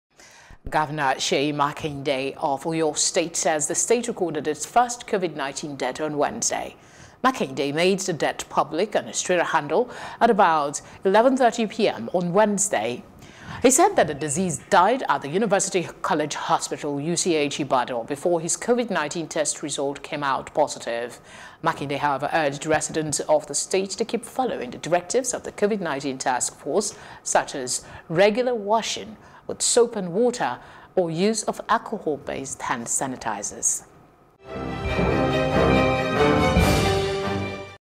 OYO RECORDS FIRST COVID 19 DEATH
Gov. Seyi Makinde of Oyo State says the state recorded its first COVID-19 death on Wednesday.